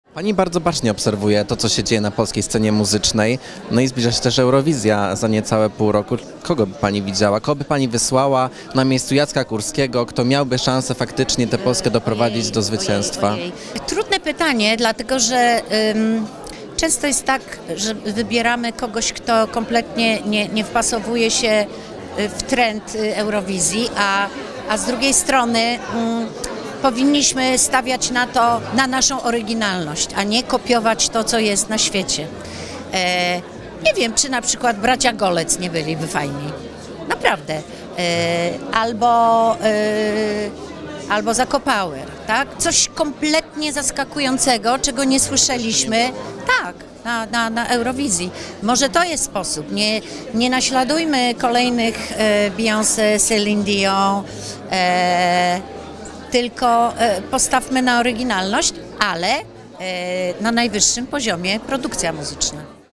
Majka Jeżowska wybrała reprezentanta Polski na Eurowizję 2019!
Kogo Majka Jeżowska wysłałby na Eurowizję 2019? Gwiazda wskazała swoich faworytów w rozmowie z naszym reporterem. Kto jej zdaniem mógłby wygrać konkurs, który w maju odbędzie się w Tel Awiwie?